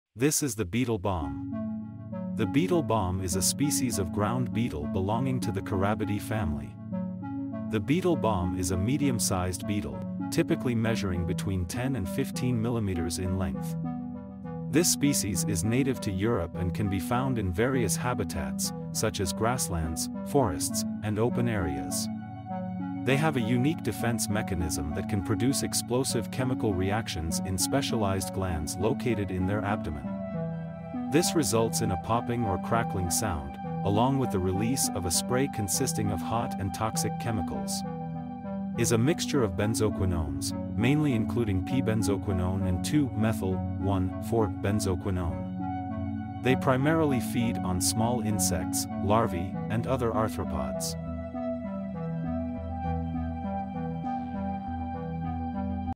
The Beetle Bomb
The beetle bomb is a species of ground beetle belonging to the Carabidae family.